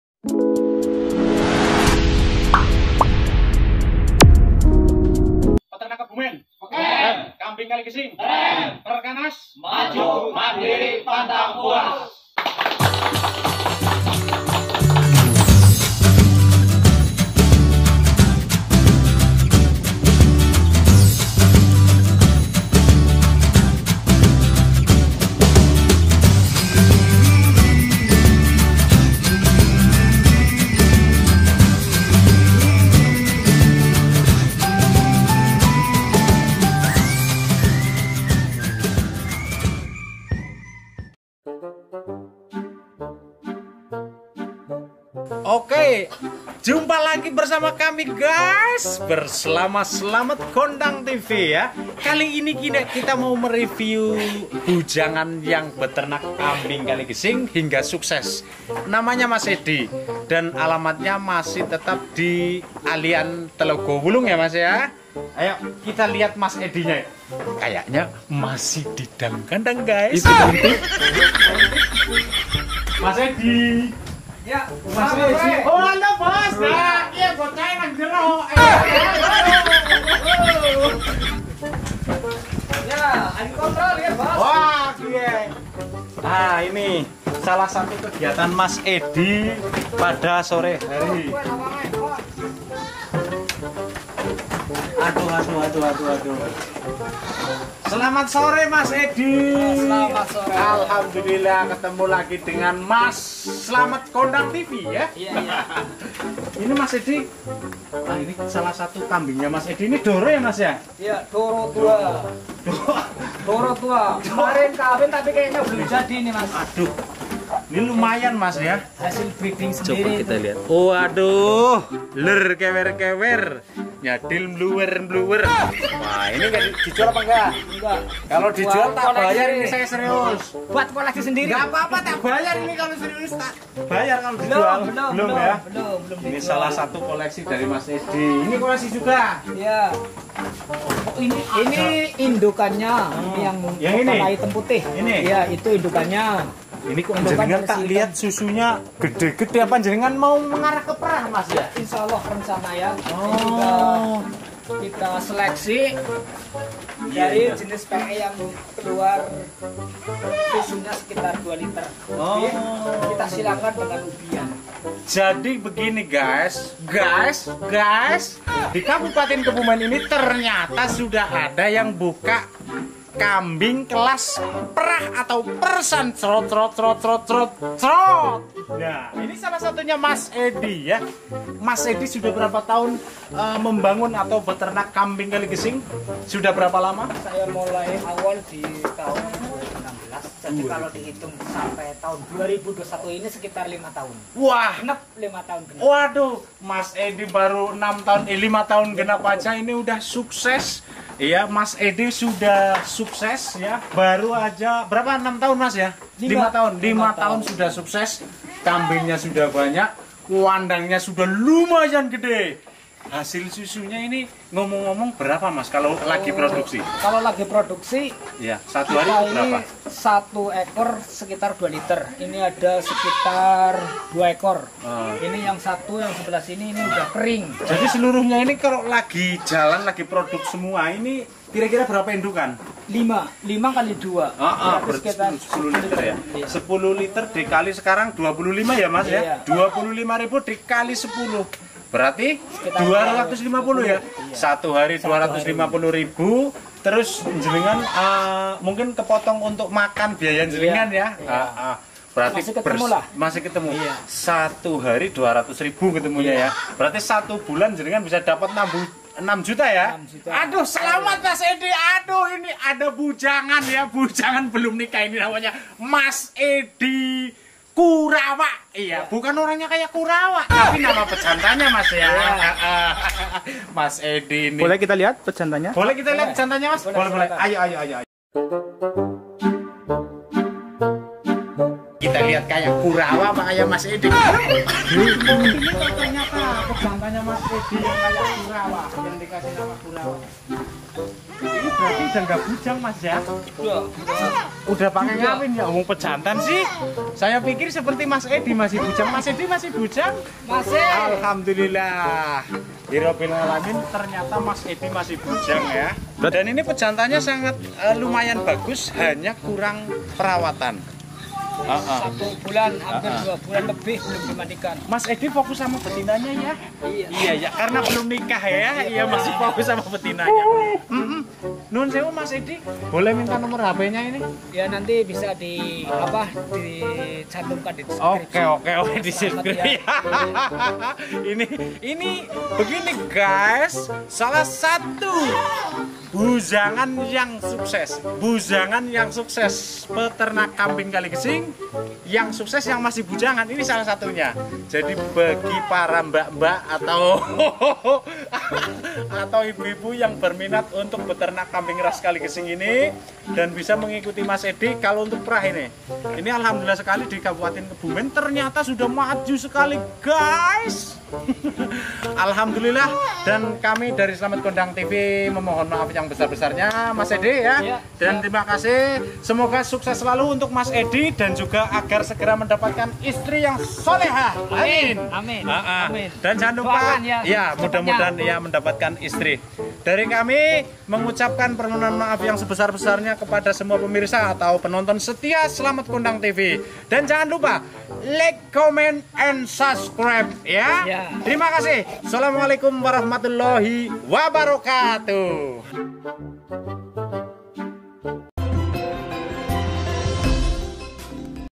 Mas Edi kurawa muda farm ll pemuda sukses beternak kambing kaligesing
Assalamualaikum peternak Indonesia,pecinta kambing etawa hari ini meliput kandang kurawa muda farm milih mas Edi,untuk alamat Desa Tlogowulung,kec.alian, kebumen,mari ikuti terus keseruanya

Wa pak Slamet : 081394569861

Maturnuwun...